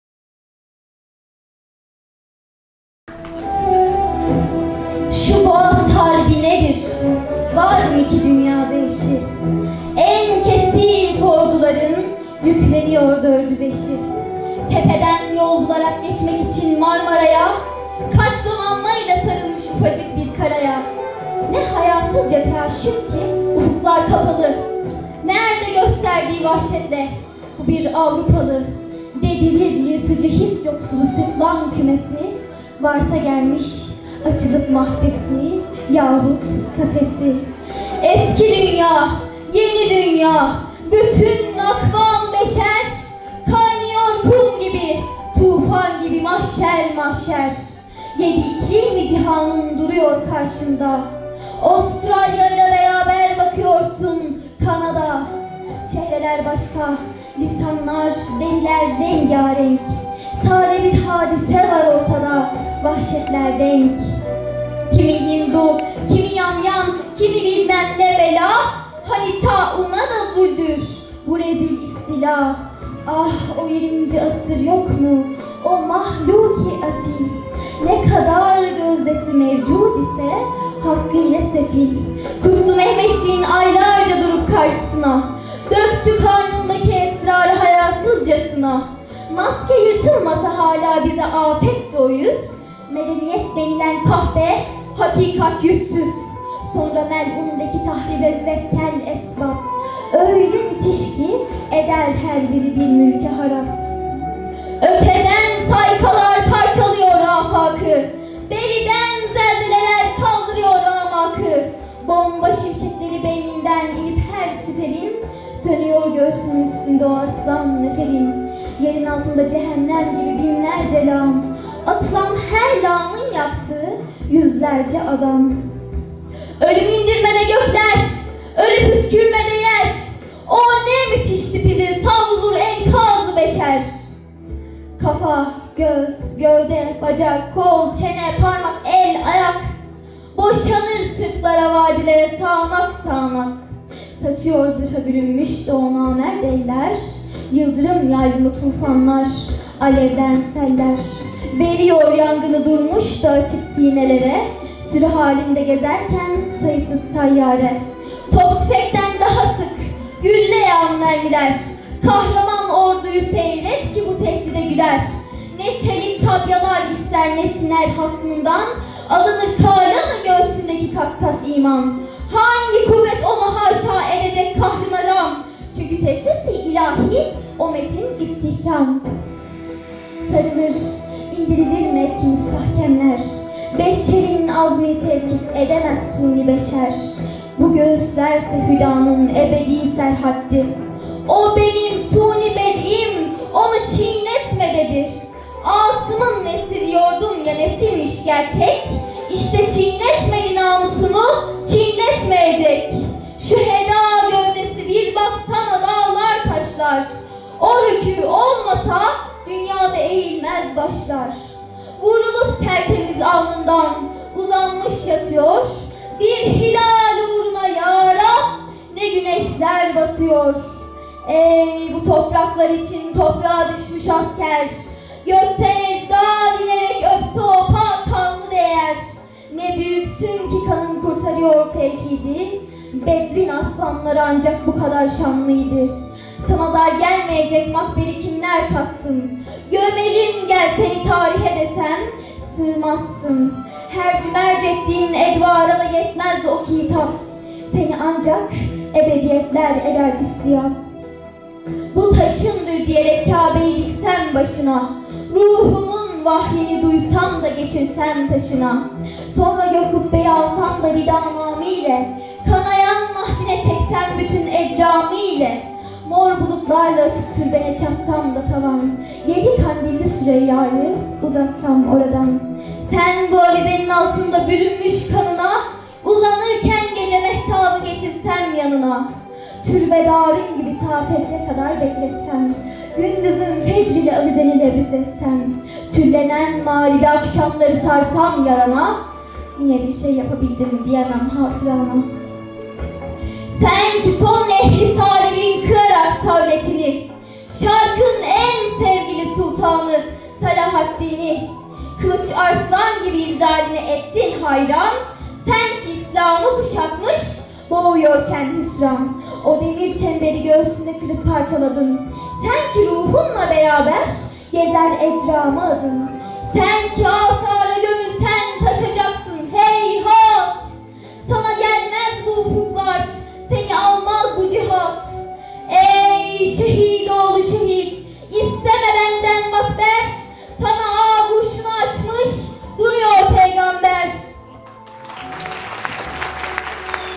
ELİF AYÇA ENGİN - ÇANAKKALE ŞEHİTLERİNE ŞİİR (İspir Anadolu Lisesi)
Mehmet Akif Ersoy - Çanakkale Şehitlerine
Seslendiren: Elif Ayça Engin 18mart çanakkalezaferi ispir erzurum şehit ispiranadolulisesi yeni #2023